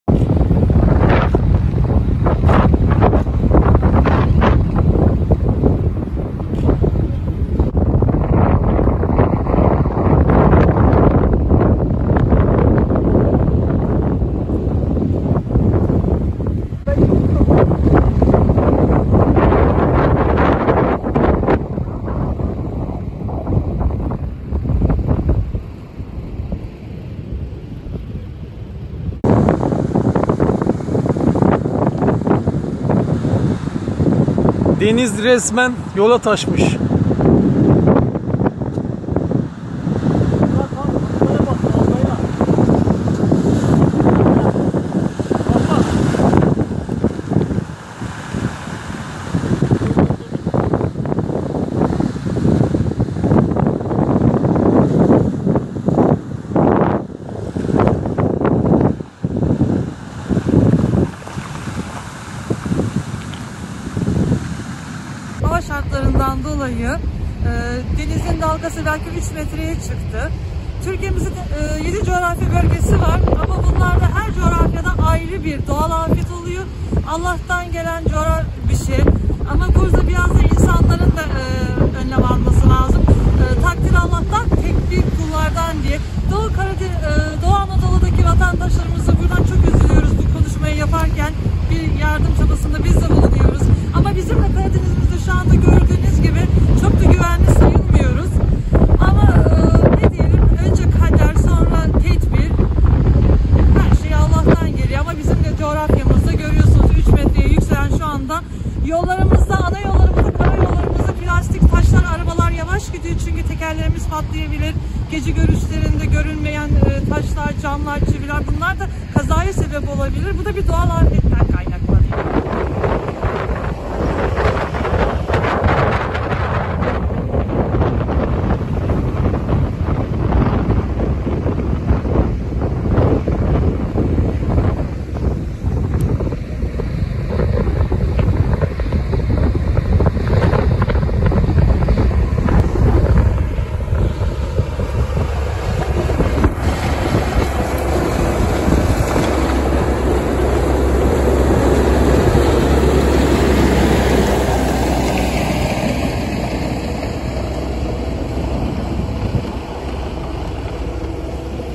DEV DALGALAR YOLA TAŞTI
fırtına nedeniyle dev dalgalar oluştu.
 Yer yer dalgaların boyunun 3 metreye ulaştığı görülürken,
 dalgaların getirdiği taşlar ve çamur bölgede trafiği olumsuz etkiledi. Sinop Havalimanı’nı kısmen su bastı.